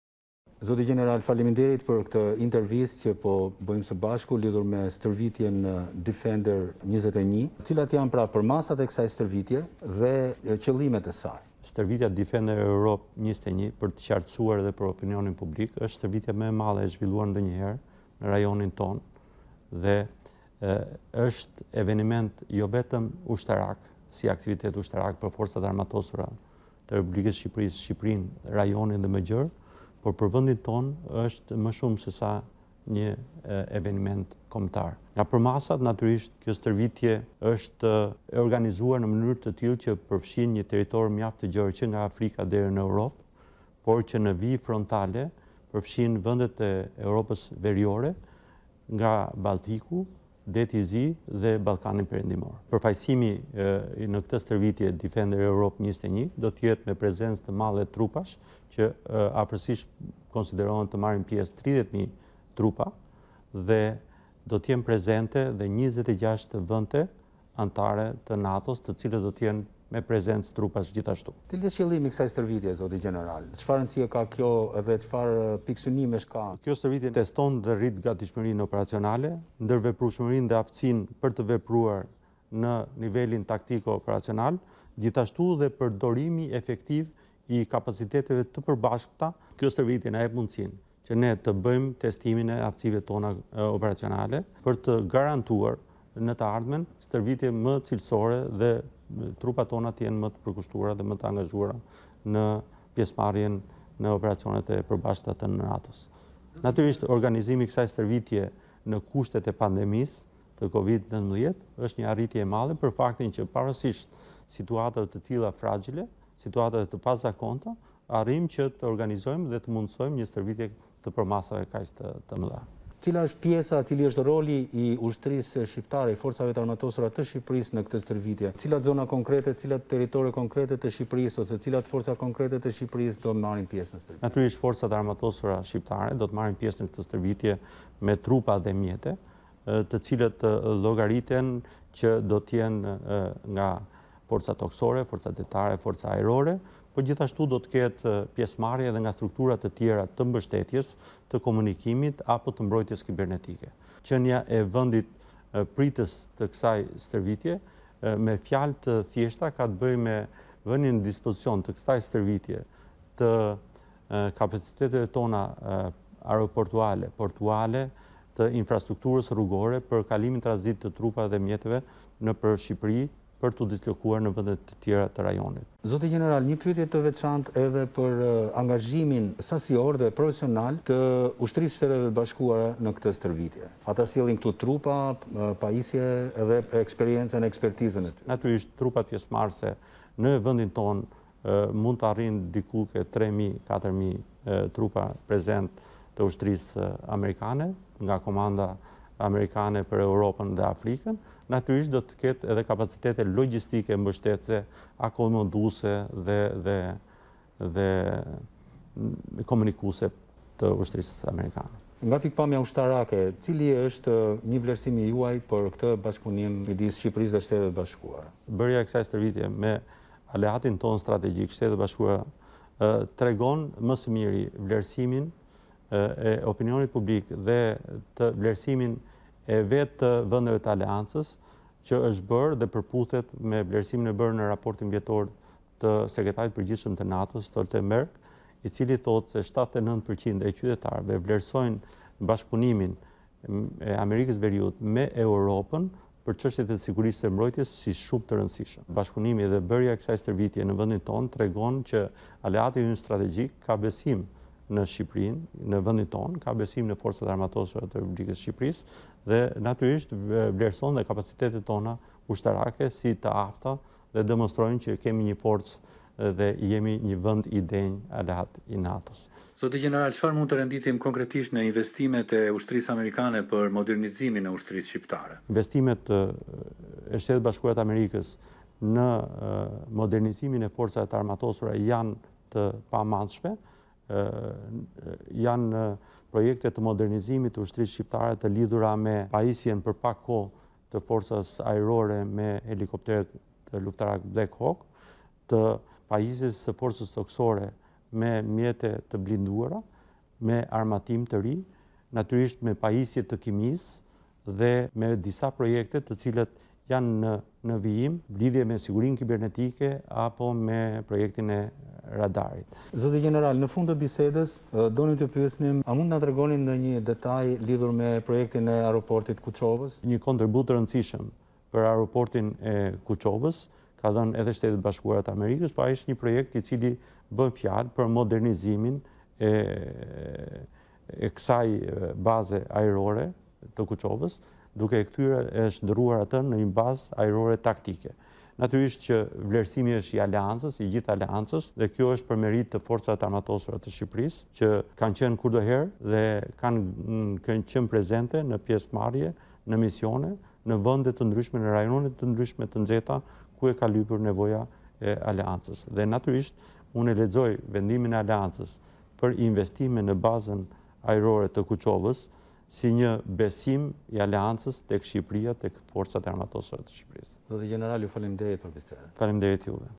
Shqipëri: Stërvitja “Defender Europe 21”, më e madhja e Aleancës. Mbi 30 mijë trupa nga 26 vende
Stërvitja “Defender Europe 21” vlerësohet si më e madhja, që është zhvilluar ndonjëherë në këtë rajon nga Aleanca. Shefi i Shtabit të Përgjithshëm të Forcave të Armatosura të Shqipërisë, Gjeneral Major Bajram Begaj, thotë se në këtë stërvitje do të marrin pjesë afro 30 mijë trupa nga 26 vende anëtare të NATO-s. Gjeneral Begaj jep më shumë detaje në një intervistë me korrespondentin tonë Ilirian Agolli, për stërvitjen dhe investimet amerikane për ushtrinë shqiptare.